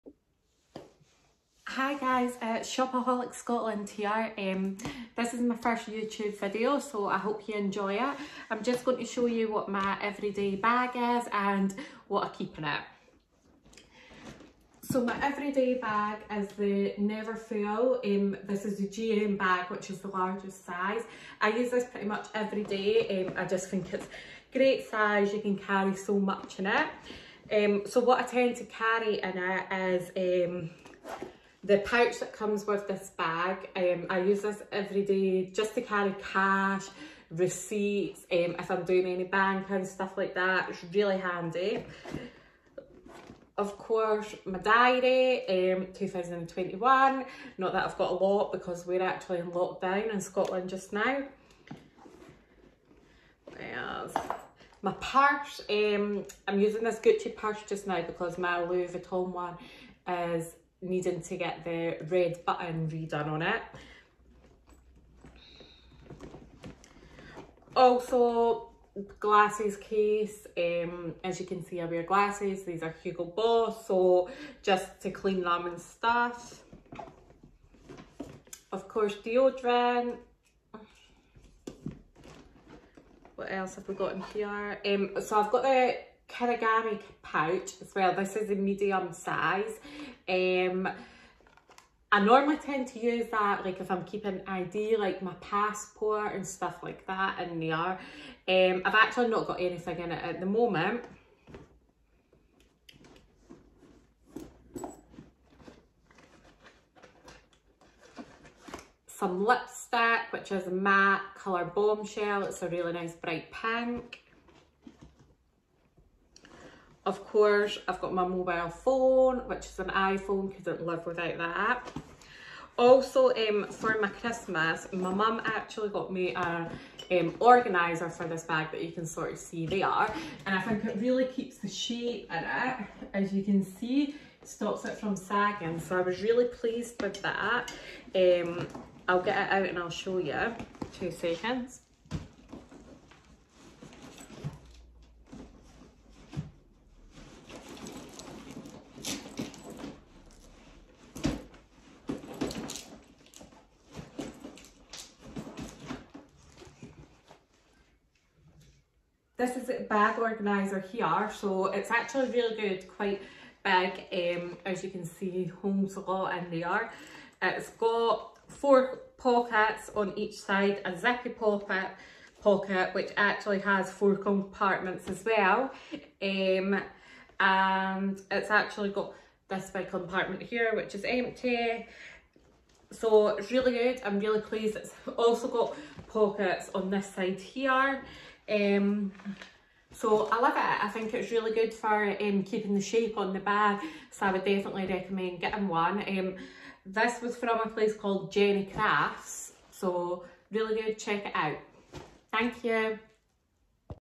My everyday handbag & what I carry
ShopaholicScotlands first YouTube video.. what I use as my everyday handbag & what I carry in it.